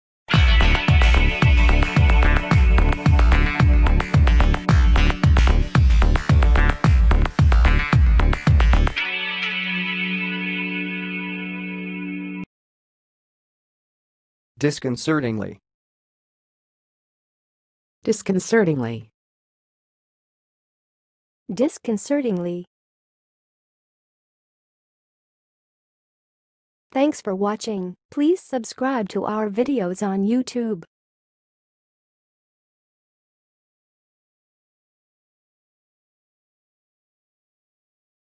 How to say "disconcertingly"! (High Quality Voices)
Watch in this video how to say and pronounce "disconcertingly"!